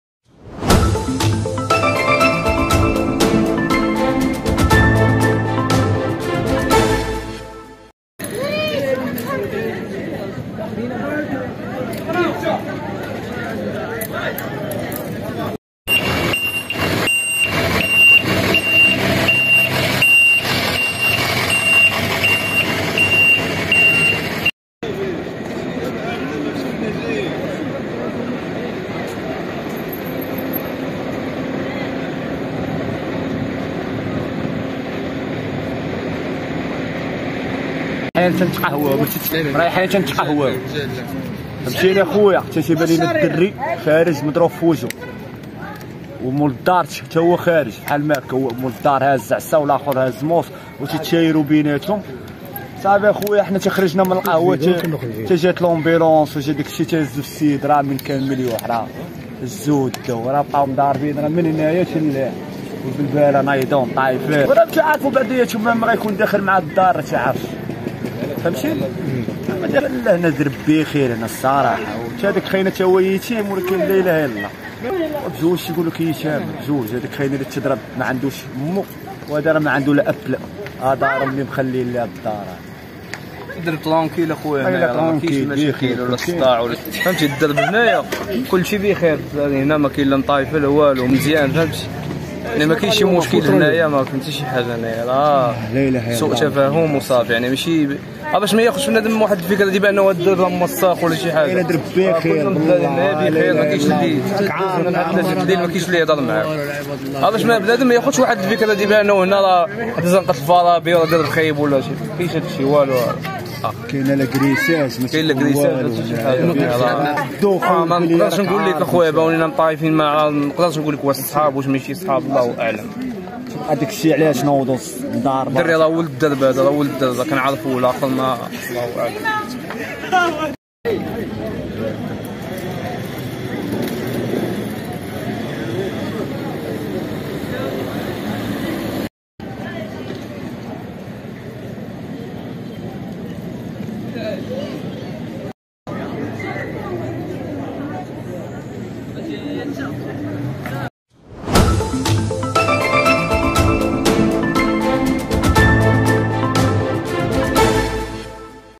جريمة قتل بالمشروع في الحي المحمدي.. نزاع بين شابين يفضي إلى مأساة
جريمة قتل بالمشروع الحي محمدي..  نزاع بين شابين يفضي إلى مأساة

# أخبار_عاجلة